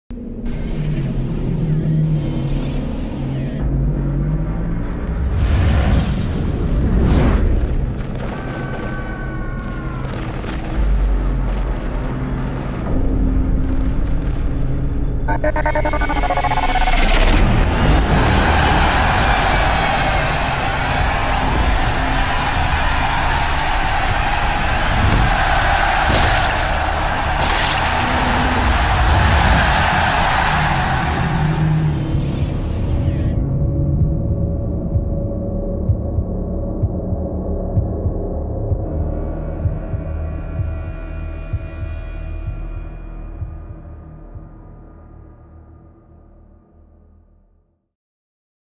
Neymar Jr in RED CARD
Check out Neymar Jr in real life making an appearance in his Red Card Zombie comic!